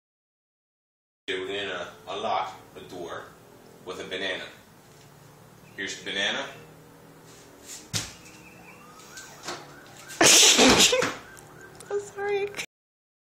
Car Hack: Unlock Any Car with A Banana!
Locked out? No problem! Use a simple trick to unlock any Before 2007 car with a banana!